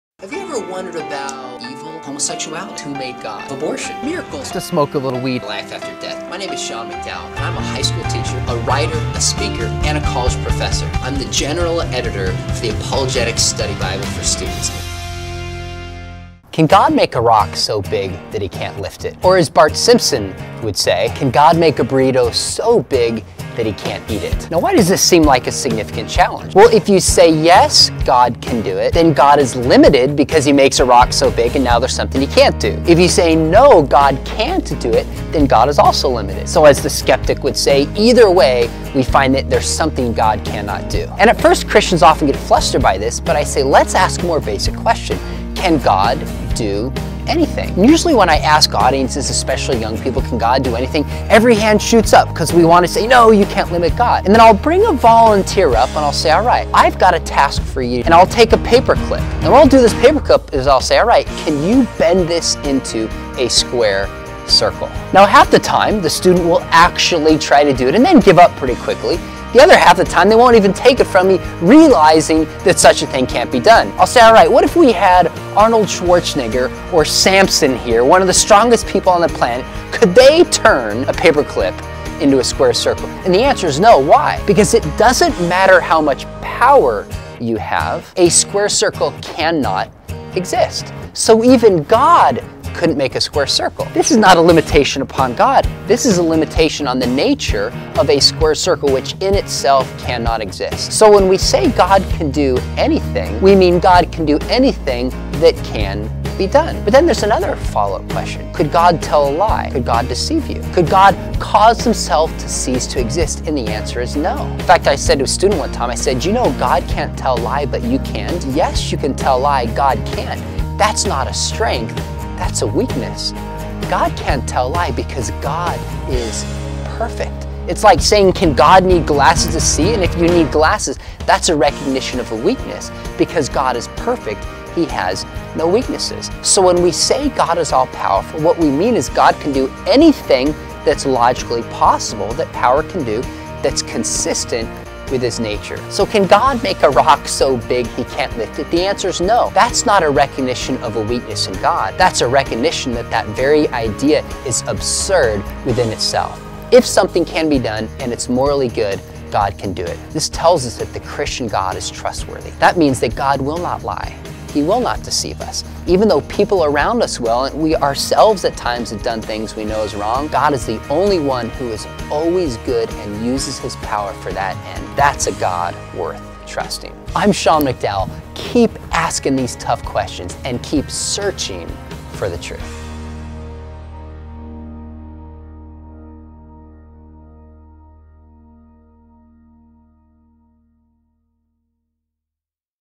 WEEK 5: Can God make a rock so big He can't lift it?
When speaking of God’s omnipotence, Christians are not claiming, “God can do anything at all.” Instead, omnipotence refers to God’s power to do anything which is both consistent with His holy character and logically possible. The “big rock” question asks if God can do something logically impossible. His inability to do such a thing does not count against His power. For example, God cannot make a square circle. So what? It’s a nonsense task. Likewise, it is a logical impossibility (and a nonsense thing to ask) for God to make a rock so big that He can’t lift it. And since God always acts in ways that are consistent with His holy nature, His inability to lie or sin does not count against His omnipotence either (Nm 23:19; Ti 1:2; Jms 1:13). The things that are true about His character will always be true, and He cannot do anything to contradict them.